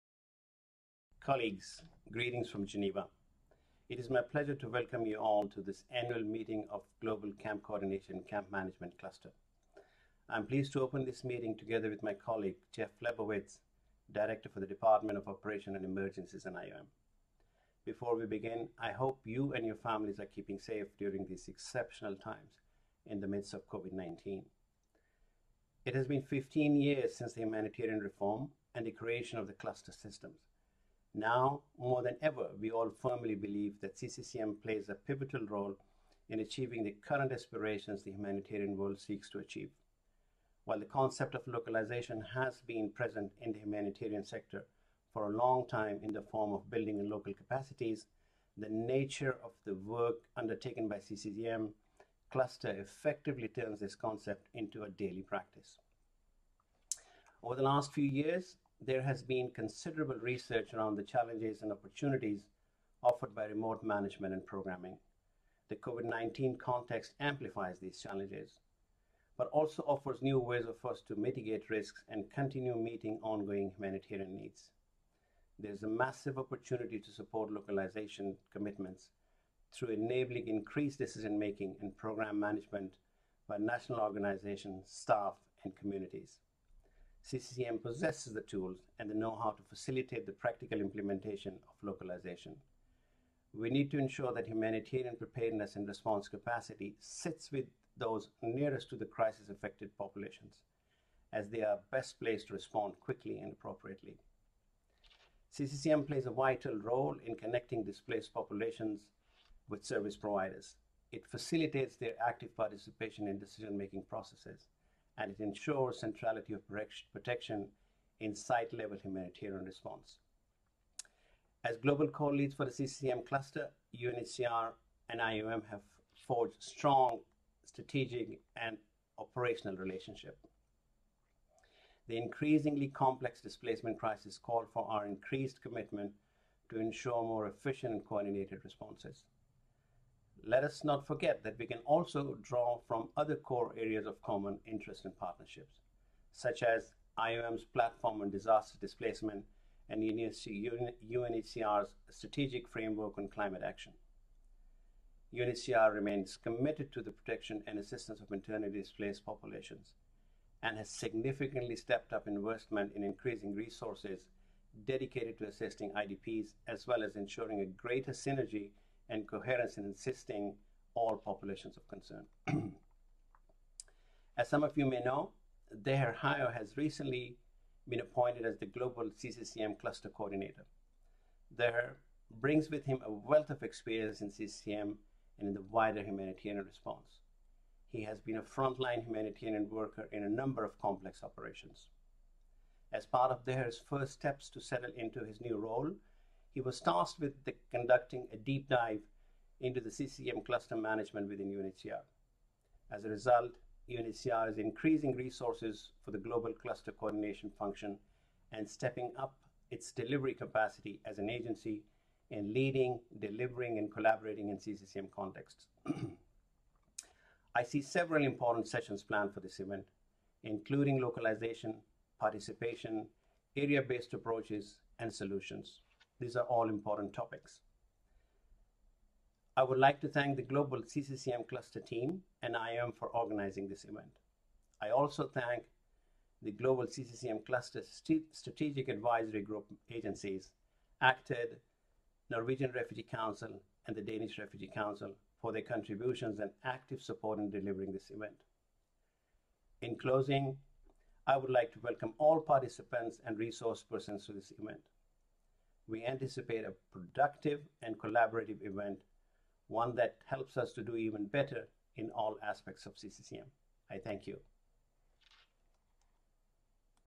Opening Remarks - Mr Sajjad Malik - Director, Division of Resilience and Solutions - UNHCR
Opening Remarks at the Global CCCM Cluster Annual Meeting 2020 - Mr Sajjad Malik - Director, Division of Resilience and Solutions - UNHCR